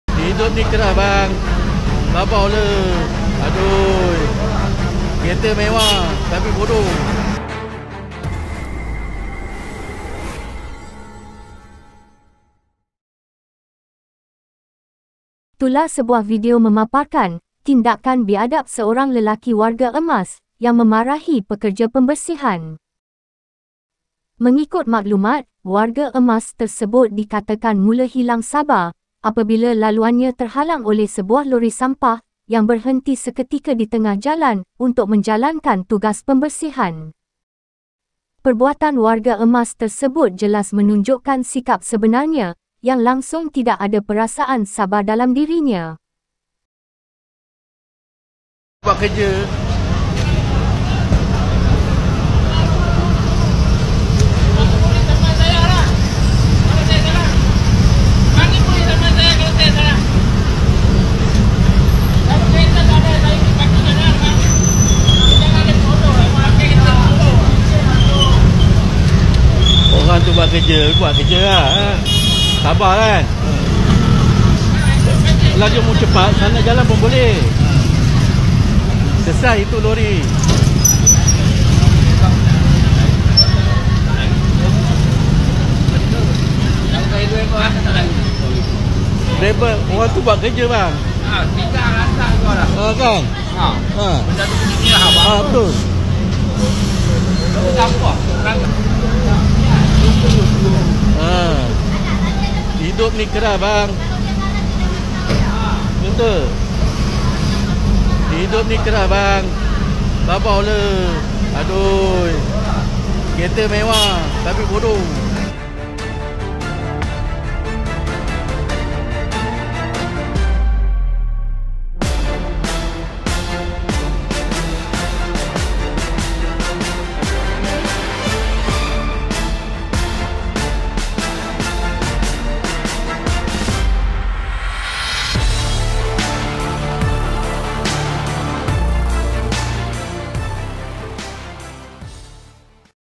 PEKERJA LORI SAMPAH KENA MARAH DENGAN WARGA EMAS
PEKERJA LORI SAMPAH KENA MARAH DENGAN WARGA EMAS memaparkan tindakan seorang warga emas yang memarahi pekerja lori sampah kerana menjalankan kerja mereka di tengah jalan